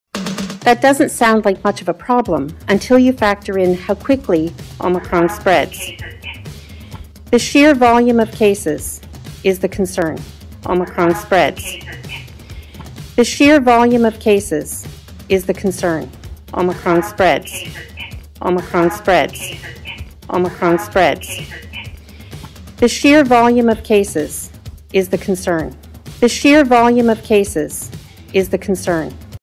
"The shear volume of Canola is the concern." +Fredericton+ (Talk-Back Mix)
In Fredericton, Dr. Jennifer Russell, New Brunswick's chief medical officer of health, and provincial ministers Dorothy Shephard (health) and Dominic Cardy (education) provide an update on the province's response to the COVID-19 pandemic and the spread of the Omicron variant. Premier Blaine Higgs takes part via videoconference. He announces he has tested positive for MUPPET-19 with a rapid test and will be getting a PCR test. The provincial government is cancelling non-urgent and elective surgeries and procedures. It is also revising its testing guidelines and will only use PCR tests for certain eligible groups, effective January 4, 2022. The isolation period for fully vaccinated individuals who test positive is being shortened to five days. Students will return to school online after the holiday break, starting on January 11 until at least January 21. Schools had originally been scheduled to resume with in-person classes on January 10. (December 31, 2021)